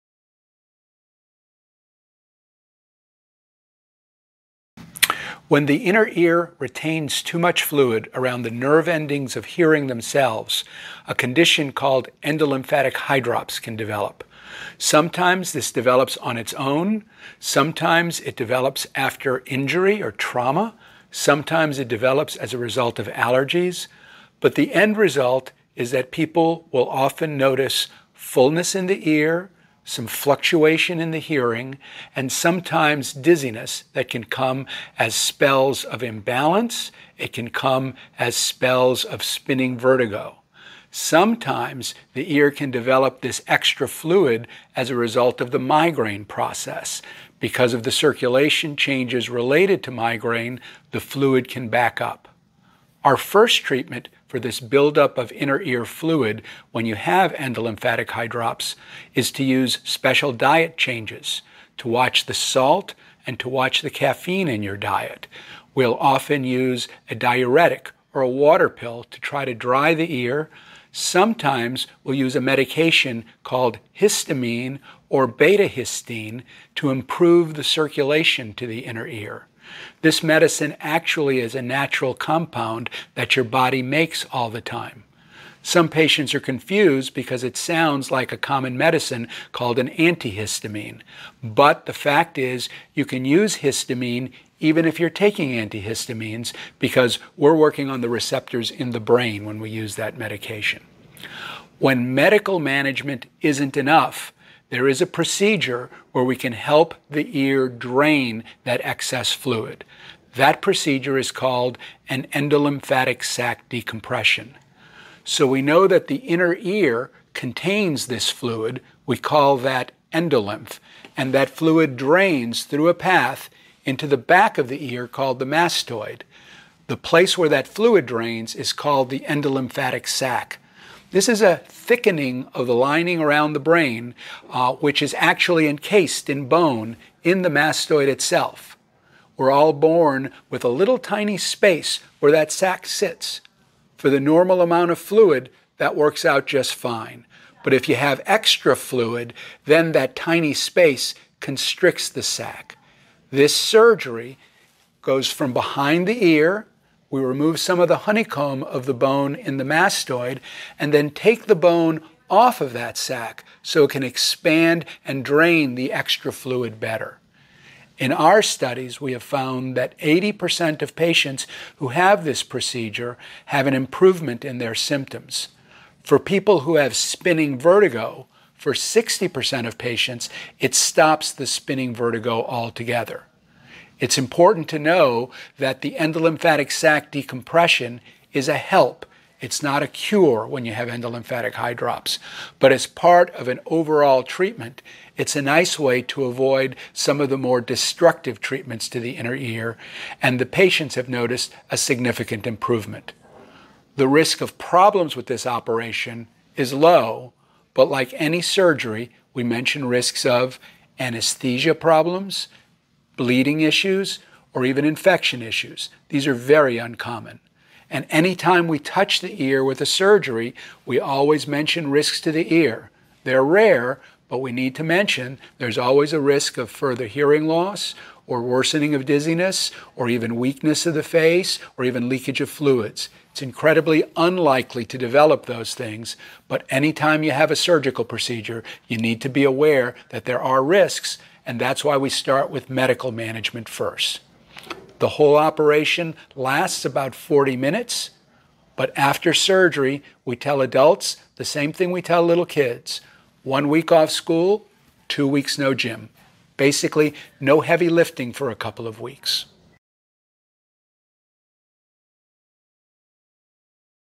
Endolymphatic Hydrops
Neurotologist Moises Arriaga of CNC Hearing and Balance Center discusses Endolymphatic Hydrops and Endolymphatic Sac Decompression.